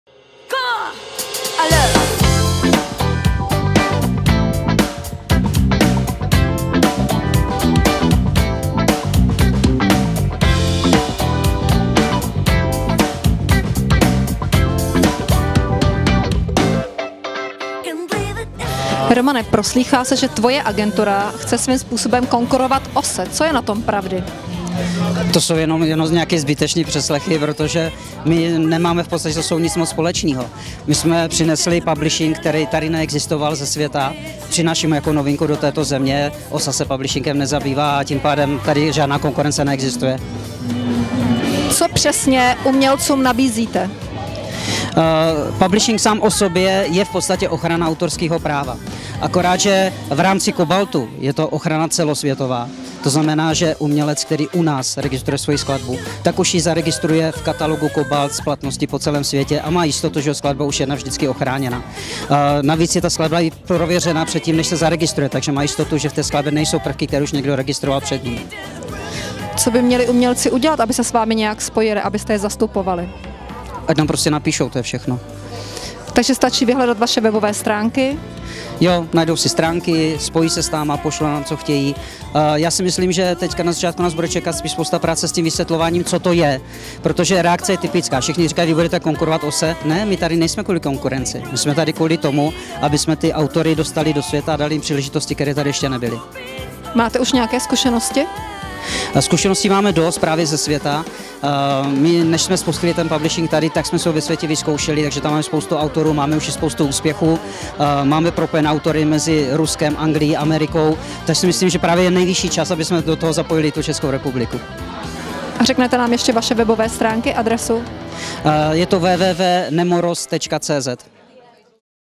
Nemoros bude zajímat hudebníky, autory, zpěváky i kapely. Poslechněte si to.
Na párty agentury Nemoros se dělo všelico. Diskuze se ovšem nesla kolem zahraničního publishingu agentury Nemoros. To, co nabízí hudebníkům, je totiž velkorysé!